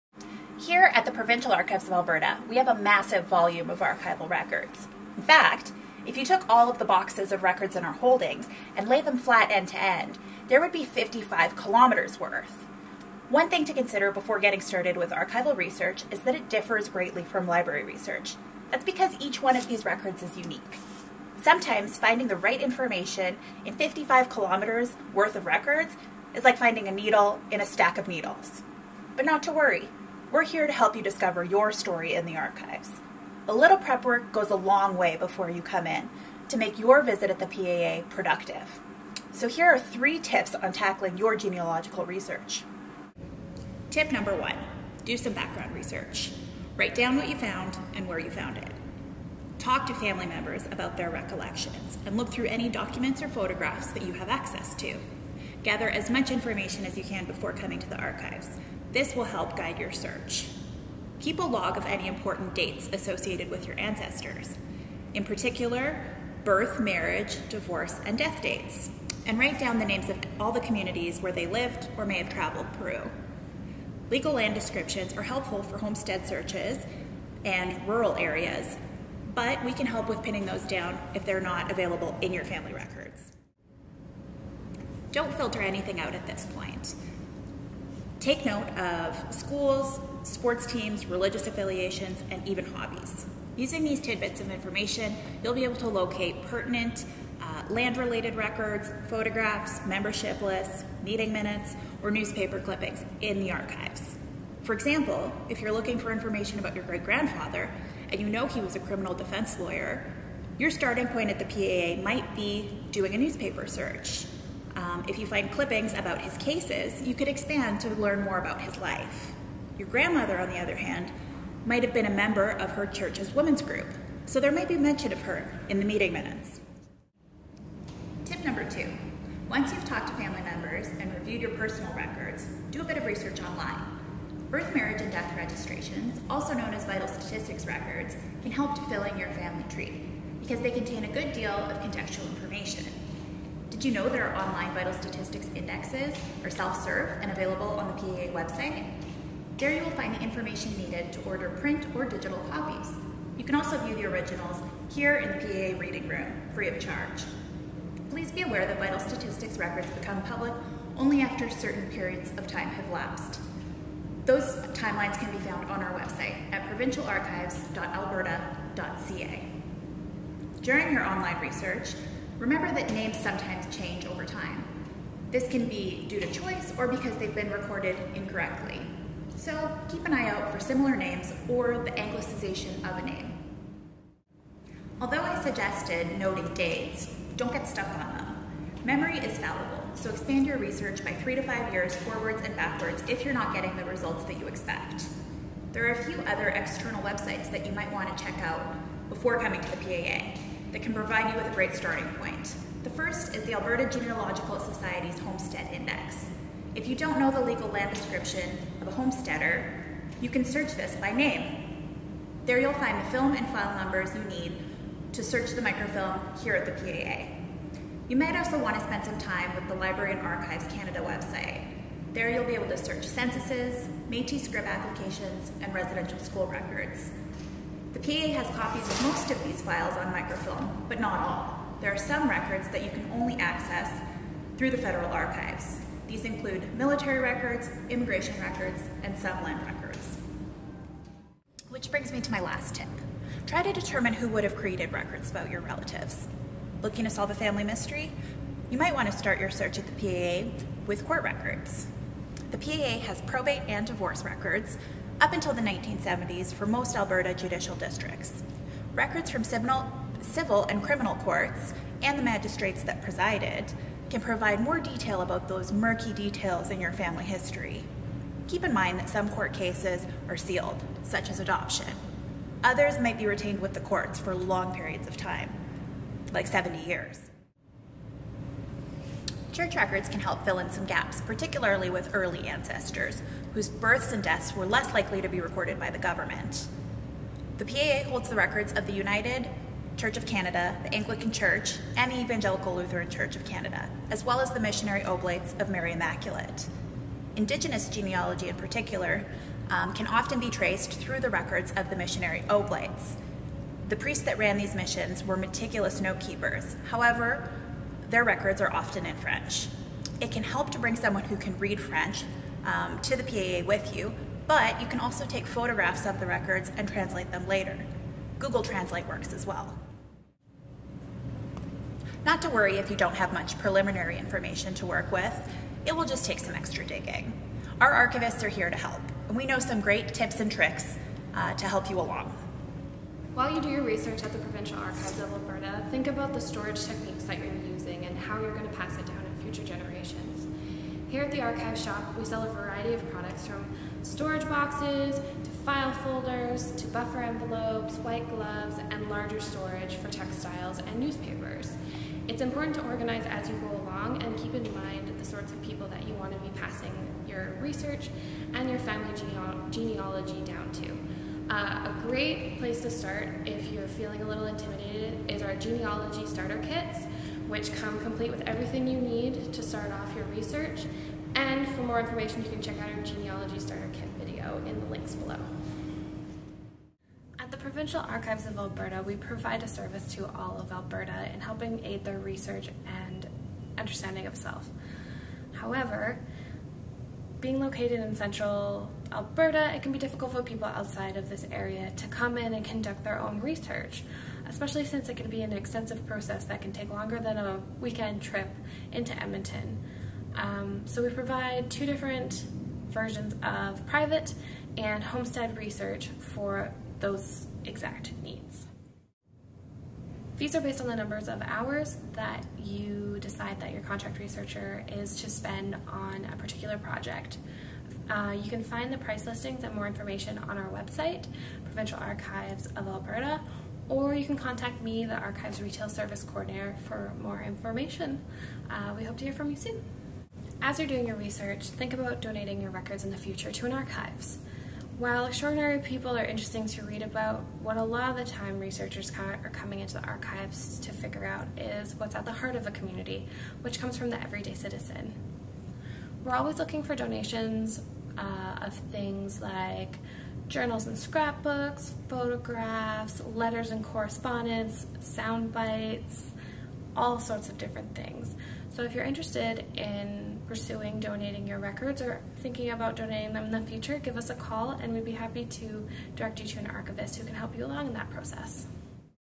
Ask An Archivist
Here at the Provincial Archives of Alberta (PAA) in Edmonton, Alberta, Canada we have over 55 kilometres of archival records that can be an incredible resource to assist you in your family genealogical research.  Archival research differs from library research as each archival record and collection is unique.  This introductory video provides researchers with several tips and resources to help you prepare for researching your family history. In advance of your visit to the Provincial Archives of Alberta, researchers are encouraged to do some background research and gather as much information as you can by speaking with family members and reviewing records that you may already have access to in your family collections.

Researchers are also encouraged to keep a log of these details noting names, dates, and locations.  Do not filter anything out at this time.  Vital statistics document birth, deaths, and marriages in the province of Alberta.  These records are available at the Provincial Archives of Alberta and are a rich resource to aid you in your research. Historical vital statistics reproductions can be ordered Additional links that can aid you in your genealogical research include the Alberta Genealogical Society Homestead Index and Library and Archives Canada

Examples of this include community organizations, school, and church records.  The Provincial Archives of Alberta holds the records of the United, Anglican, and Evangelical Lutheran Churches of Canada, as well as the Missionary Oblates of Mary Immaculate (OMI). Reference archivists at the Provincial Archives of Alberta are here to help you find what you need.

If you are a family historian, consider how you will store and pass down your records and research to future generations.  The Archives Store, located at the Provincial Archives of Alberta, sells archival and museum-quality preservation supplies and storage boxes, photo sleeves, and buffered envelopes.  In addition to archival supplies, the Archives Store also offers other client services.